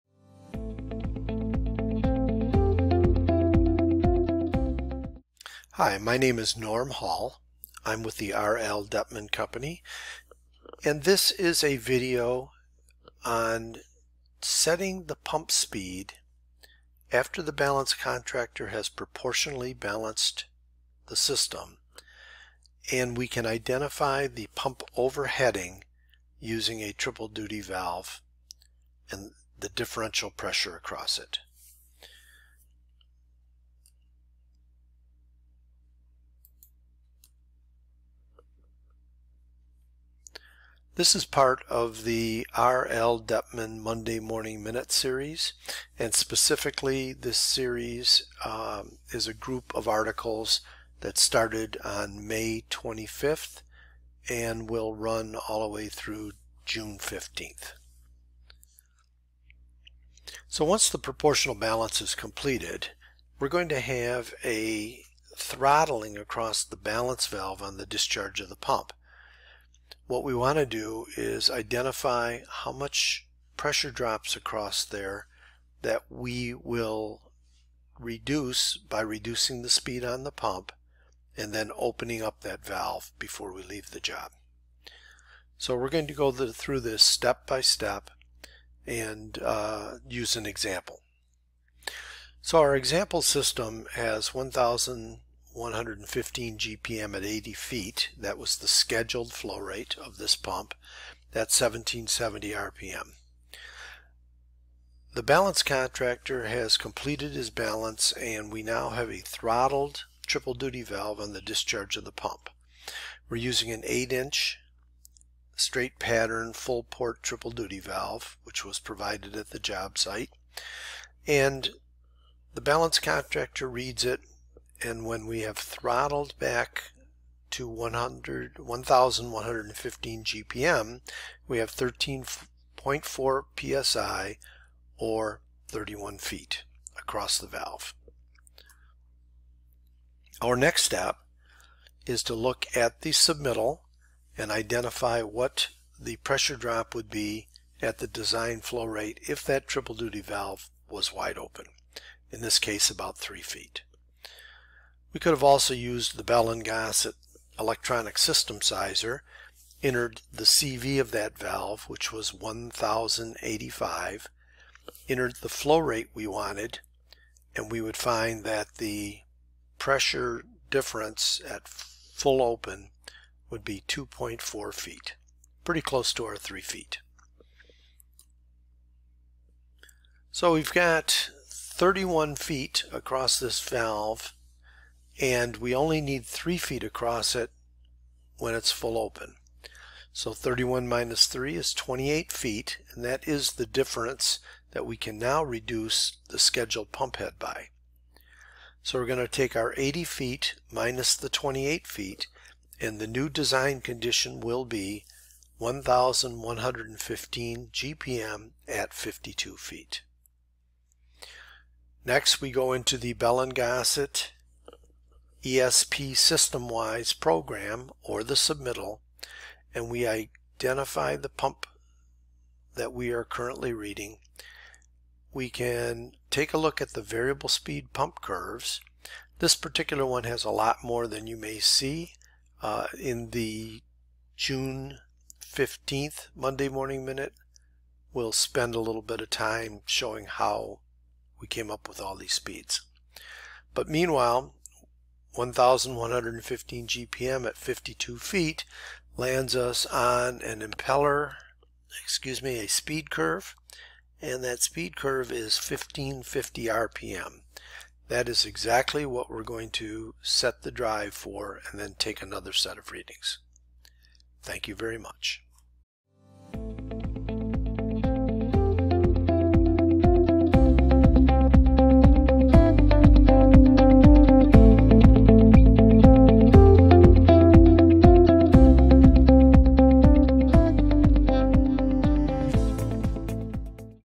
Variable Speed Pump Balance Determining the Initial Speed Setting Using A Triple Duty Valve Reading
Most centrifugal pumps in heating and cooling systems have some oversizing in the head calculation. Once the HVAC hydronic system is proportionally balanced per ASHRAE 90.1 energy standards, all this extra head is known. The VFD speed may be dialed down to save energy and complete the balance. Just what are the steps required to complete this valuable process? Watch the video to understand the steps you need to set the initial speed of a variable speed pump with a triple duty valve reading.

Learn more and see all the step to balance the hydronic system pumps in this Monday Morning Minute Article: